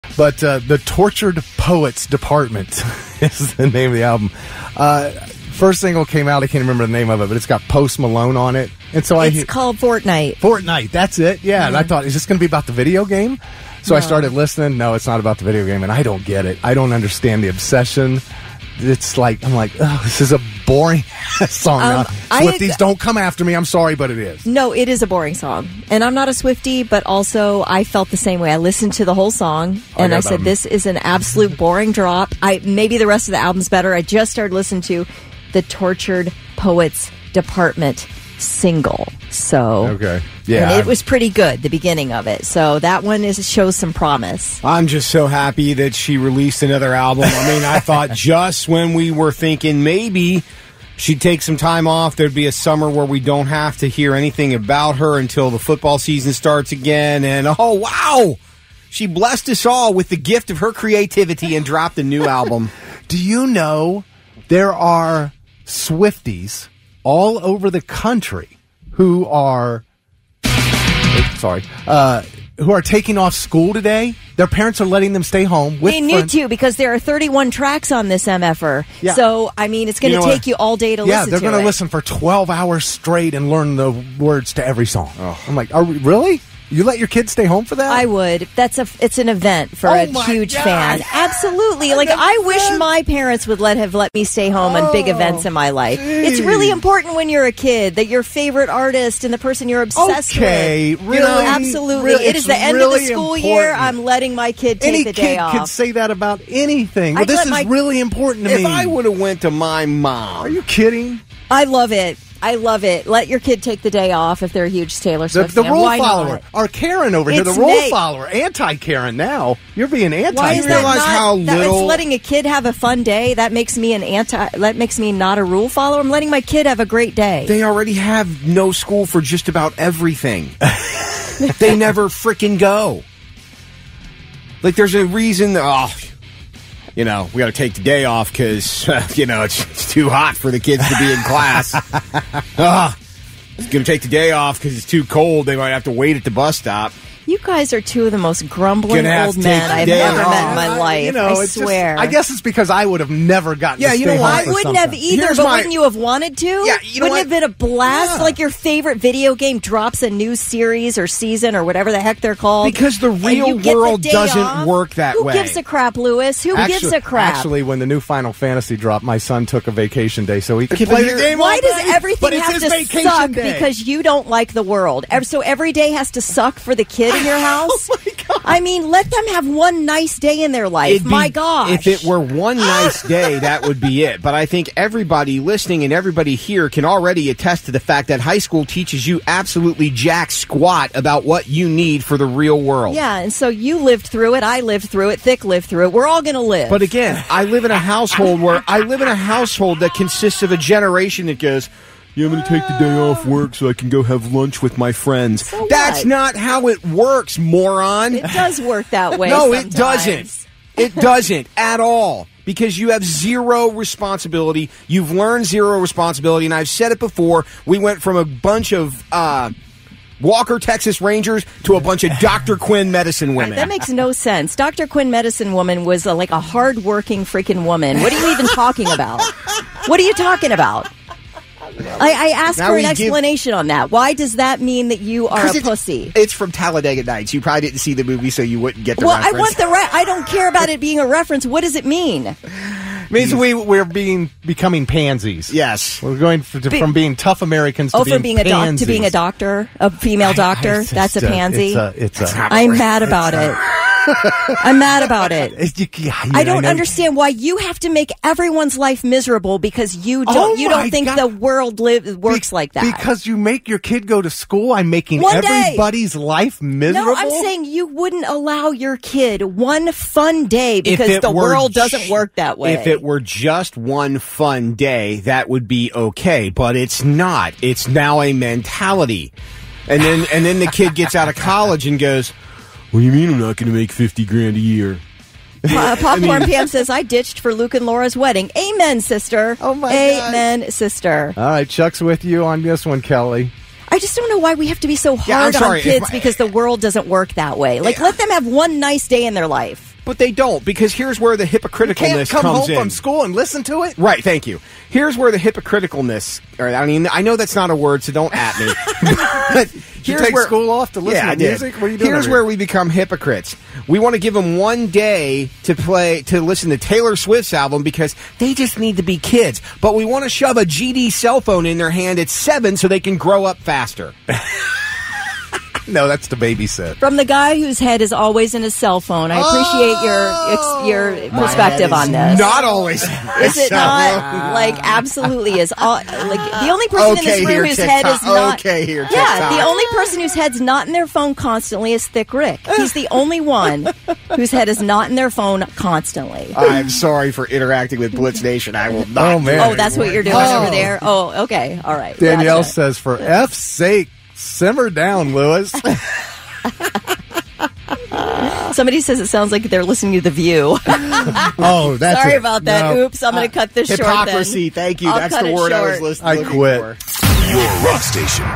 A Day Off To Listen To The New Taylor Swift Album??!!!
There are some parents giving kids the day off to listen to the new Taylor Swift album. Kelly was in love with this idea and Lewis was not. Shocker, they argued about it!